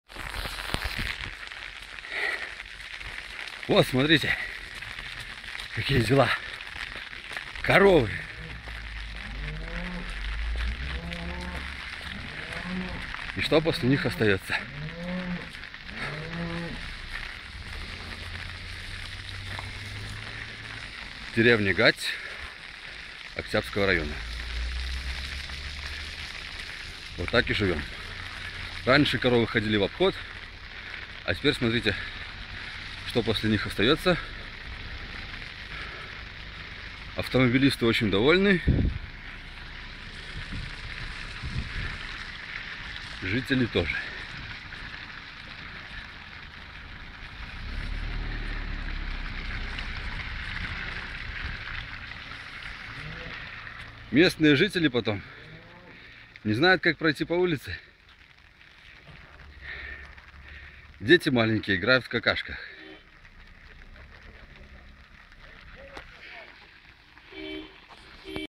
Гать. Жизнь в довне
Я не паэт, пісать не ўмею,
но вам пра эта раскажу:
Засрали Гать усю каровы
я іх за это накажу!

Калі па Гаці ты праехаў,
альбо прайшоў у сапагах -
гаўном заляпаны паўсюды
і на штанах і на вушах.

Па глаўнай вуліцы дзярэўні
і са срання і вечарком
кароў ганяюць без зупынка,
каб ям не бачыў райспалком.

Машыны людзі мыюць злобна,
Гаўно ад краскі не аддраць!
А сапагі, штаны і майкі
Прыходзіцца нам выкідаць!

Сваім стіхом хачу давесці
да райсполкома я адно,
што ўжо ў Мозыр прыязджае
на колах гацкае гаўно!